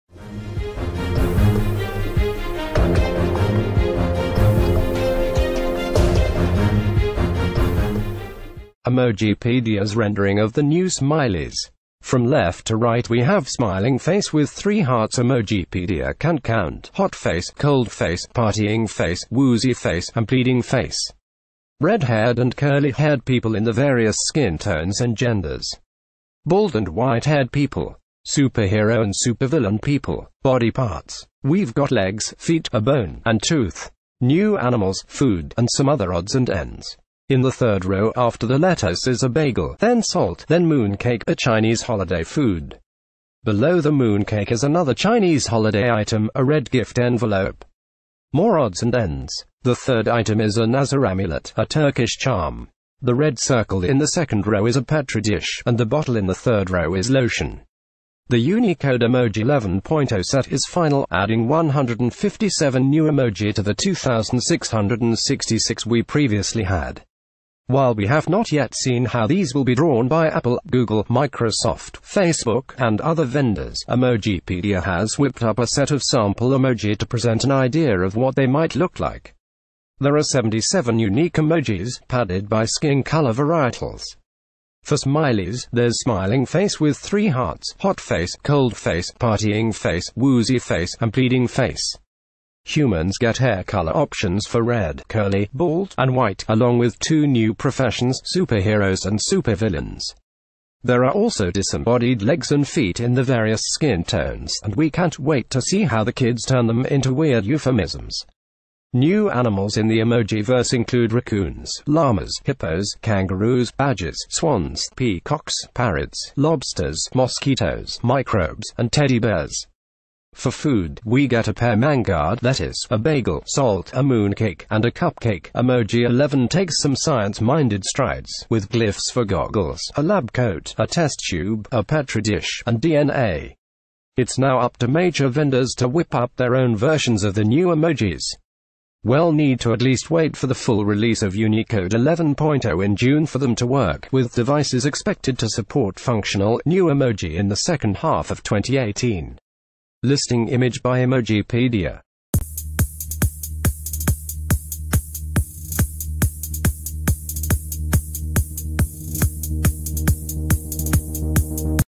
Behold, the 157 new emoji for 2018 by BuzzStyle
Emojipedia's rendering of the new smileys. From left to right we have "Smiling Face With 3 Hearts" (Emojipedia can't count), "Hot Face," "Cold Face," "Partying Face," "Woozy Face," and "Pleading Face." "Red-haired" and "curly haired" people in the various skin tones and genders. "Bald" and "white-haired" people. "Superhero" and "supervillain" people. Body parts! We've got legs, feet, a bone, and tooth. New animals, food, and some other odds and ends. In the third row, after the lettuce is a bagel, then salt, then "moon cake," a Chinese holiday food. Below the moon cake is another Chinese holiday item, a red gift envelope. More odds and ends. The third item is a "Nazar Amulet," a Turkish charm. The red circle in the second row is a petri dish, and the bottle in the third row is "lotion." The Unicode Emoji 11.0 set is final, adding 157 new emoji to the 2,666 we previously had. While we have not yet seen how these will be drawn by Apple, Google, Microsoft, Facebook, and other vendors, Emo...